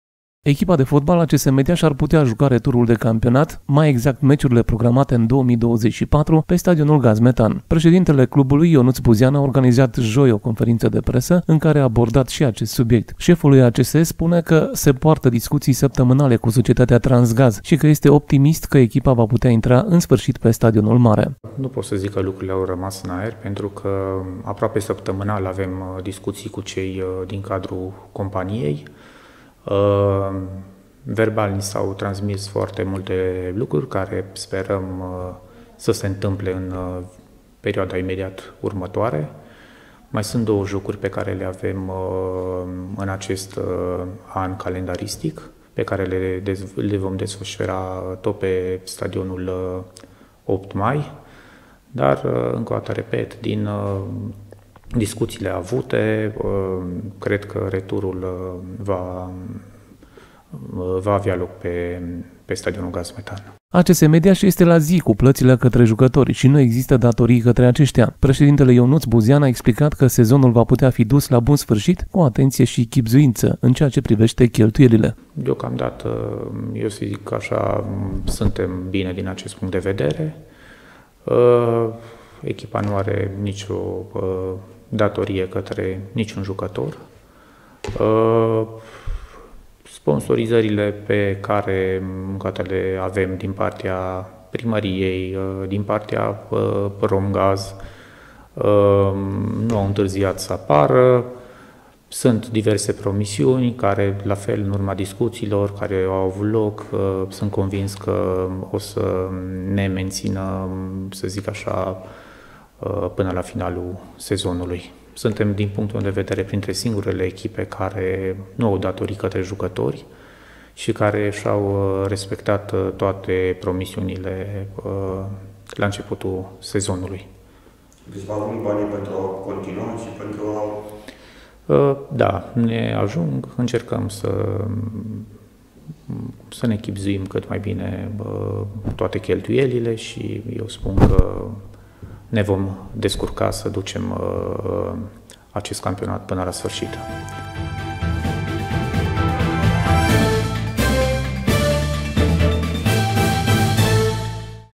ACS Mediaș pe Stadionul Gaz Metan din 2024 ? | Jucătorii sunt cu banii la zi | novatv.ro
ACS Mediaș ar putea juca returul de campionat, mai exact meciurile programate în 2024, pe Stadionul Gaz Metan. Ionuț Buzean a organizat joi o conferință de presă în care a abordat și acest subiect. Președintele clubului spune că se poartă discuții săptămânale cu societatea Transgaz și că este optimist că echipa va putea intra în sfârșit pe stadionul mare. ACS Mediaș este la zi cu plățile către jucători și nu există datorii către aceștia. Președintele Ionuț Buzean a explicat că sezonul va putea fi dus la bun sfârșit cu atenție și chibzuință în ceea ce privește cheltuielile.